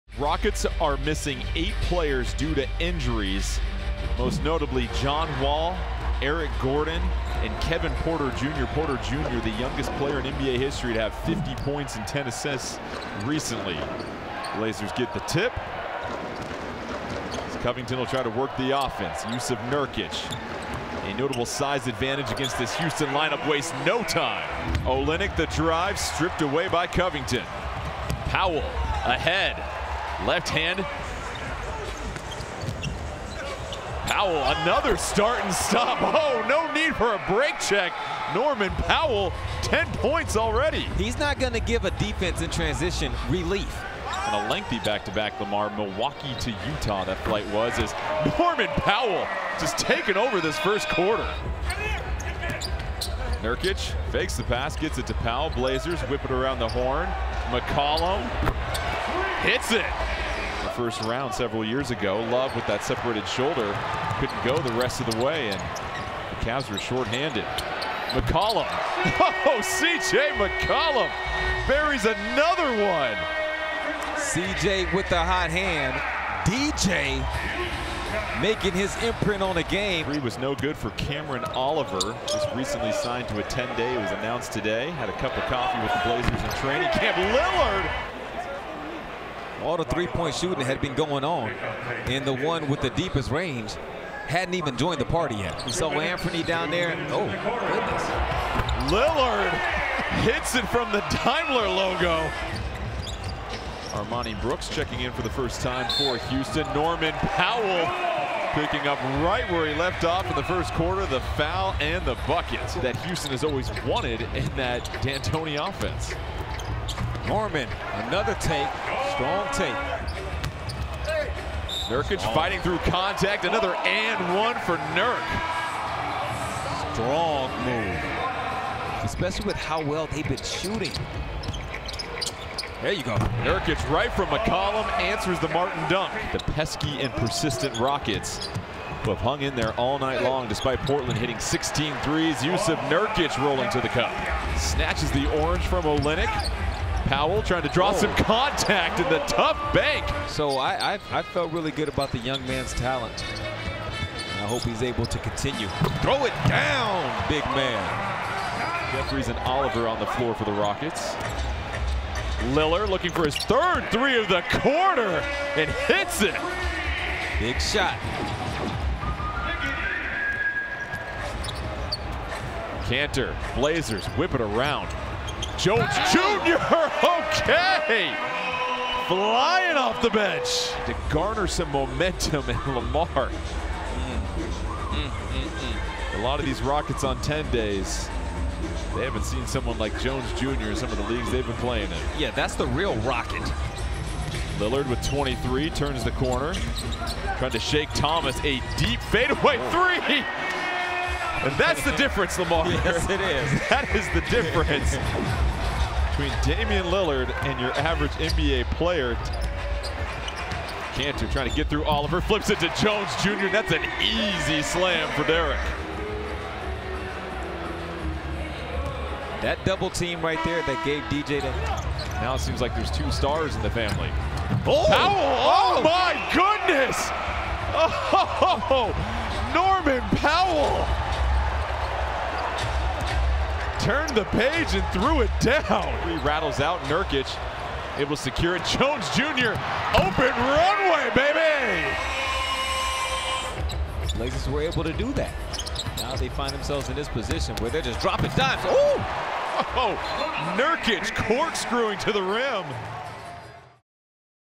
Trail Blazers 140, Rockets 129 | Game Highlights by McDelivery | May 10, 2021
Damian Lillard scored 34, Norman Powell and CJ McCollum scored 28 each, and Jusuf Nurkic scored 22 as the Blazers notched their 40th win of the season vs. Houston on Monday.